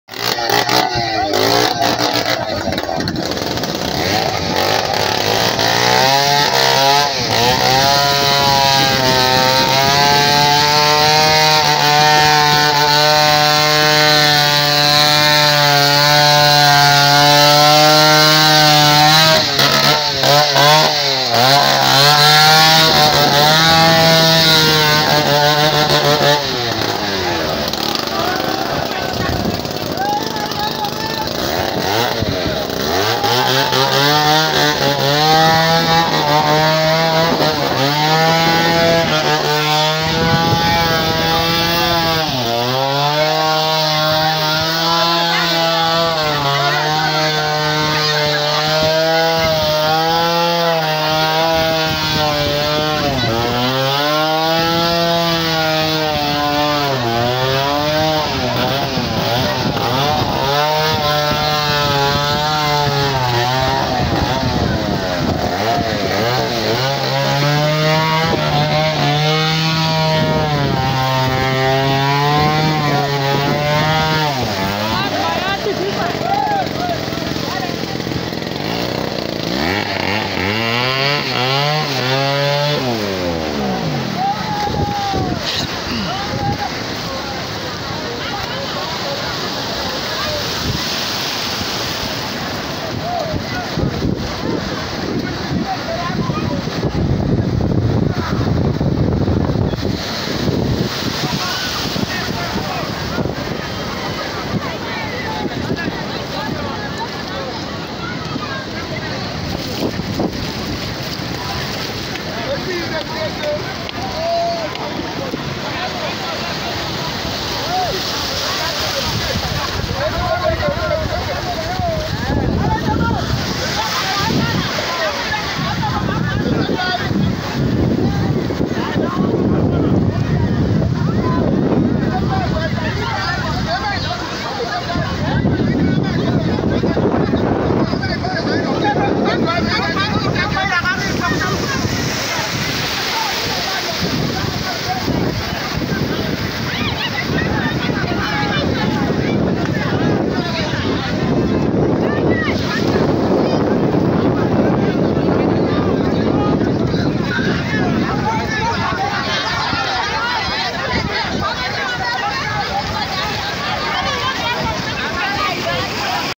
deadly consumption of a beach whale in bayelsa,nigeria
A beach Whale killed by mass numbers of villagers in Bayelsa State, Nigeria.